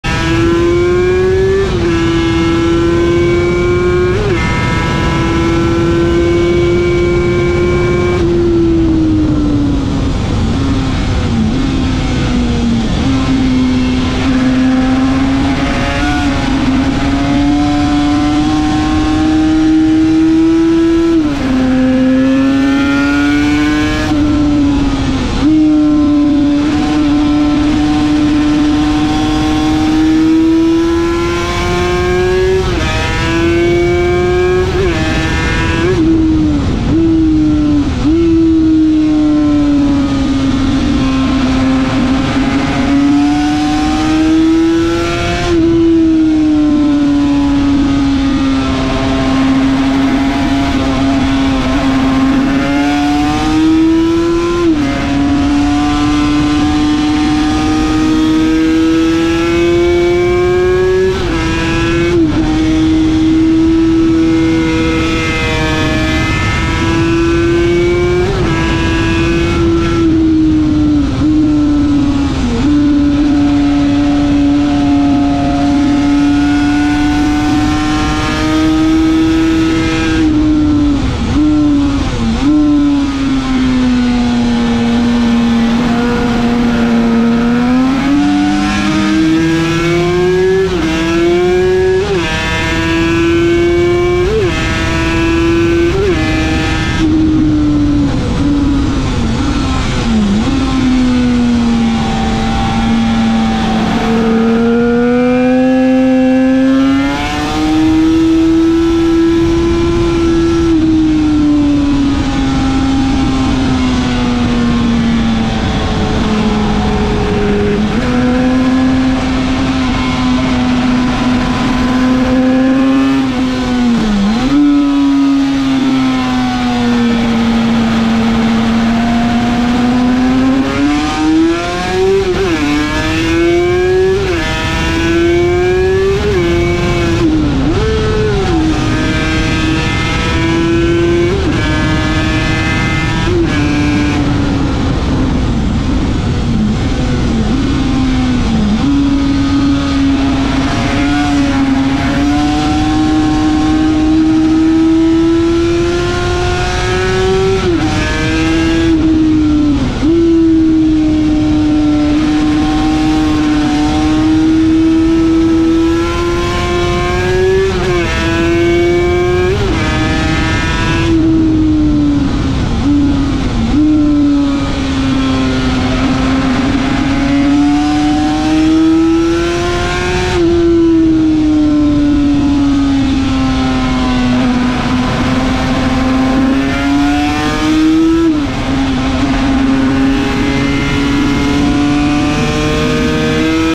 Yamaha YZR500
Norick Abe Yamaha YZR500 Replica twostroke
at TT Circuit Assen and Close call Nice save